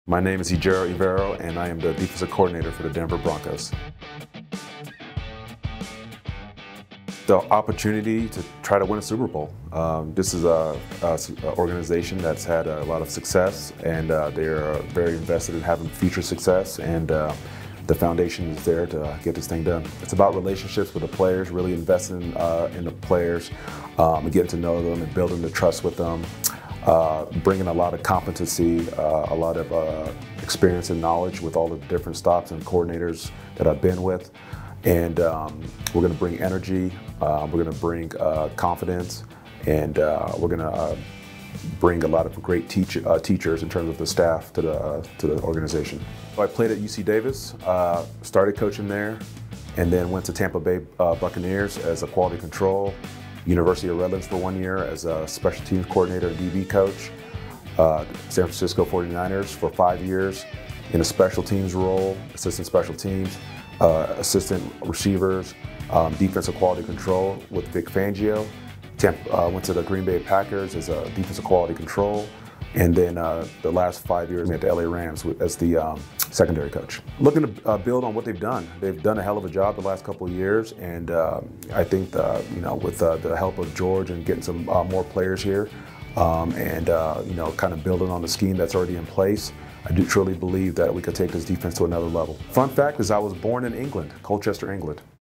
Get to know Defensive Coordinator Ejiro Evero
New Broncos Defensive Coordinator Ejiro Evero discusses his coaching philosophy, what he's looking to bring to the defense and more.